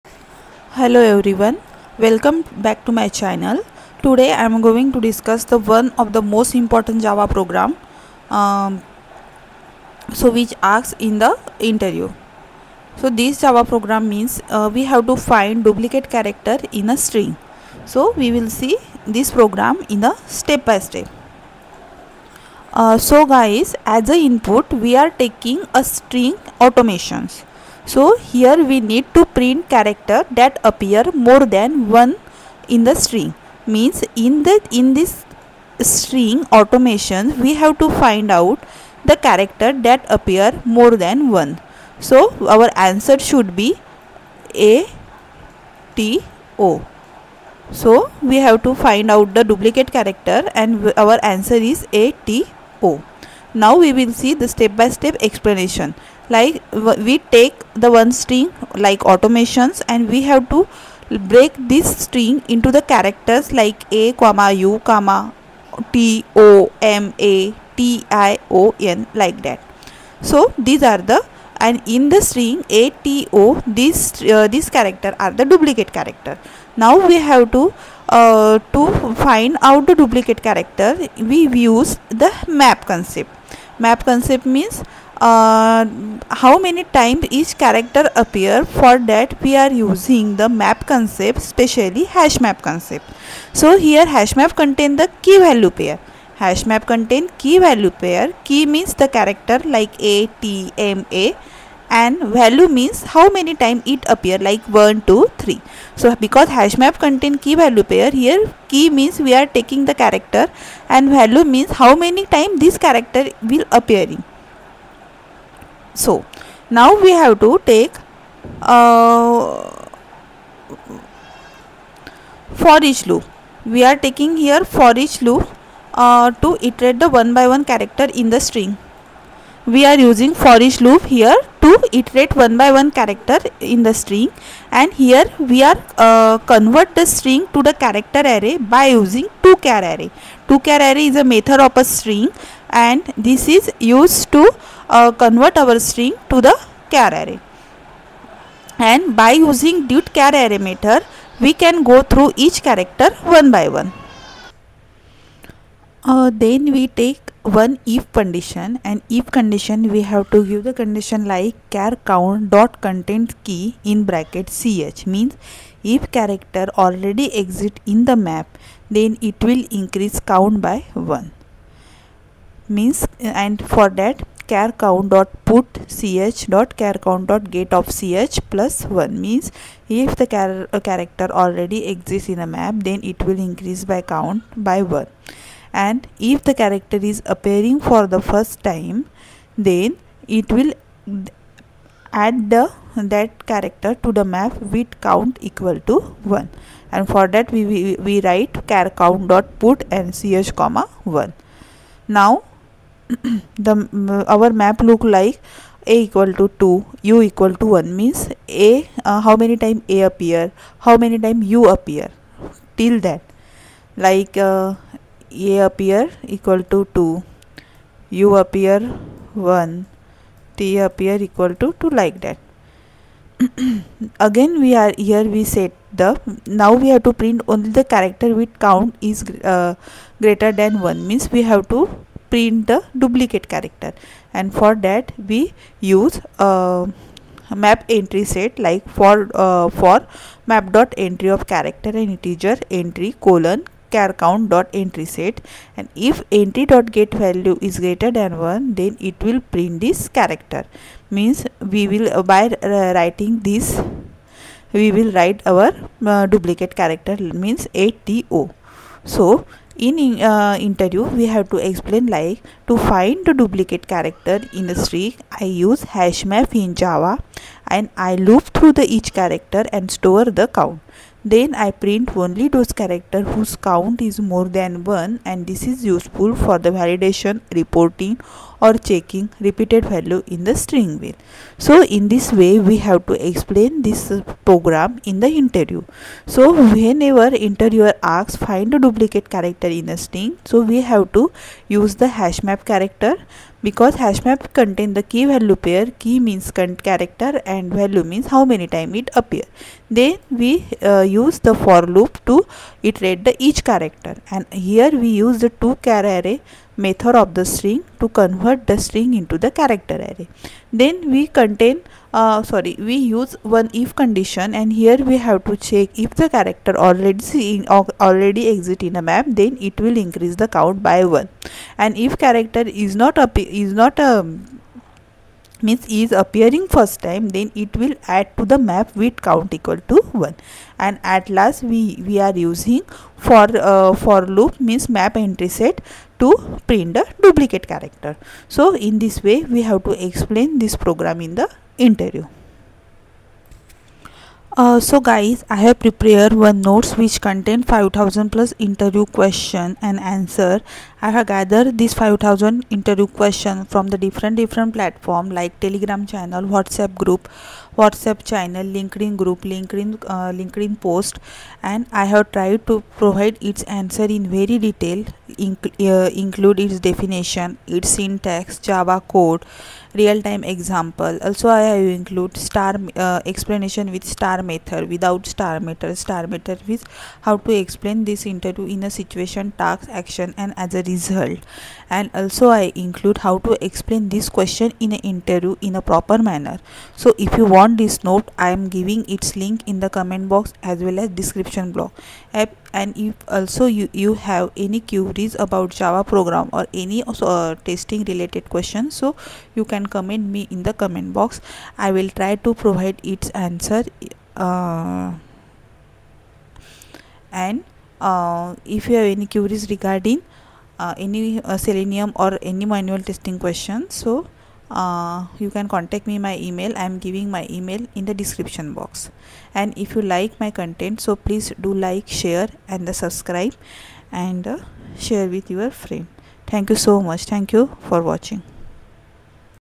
Find Duplicate Characters in String | Most Important JAVA Programs for Tester
Java Tutorial for Beginners | Learn Java Programming for Beginners

📌 In this video, I’ll show you three powerful ways to reverse a string in Java:
✅ Iterative Method (Using Loop)
✅ StringBuilder Method (Using Built-in Reverse Function)

🔥 Whether you're preparing for Java interviews, improving your coding skills, or just exploring different approaches, this video will help you understand and master string reversal efficiently!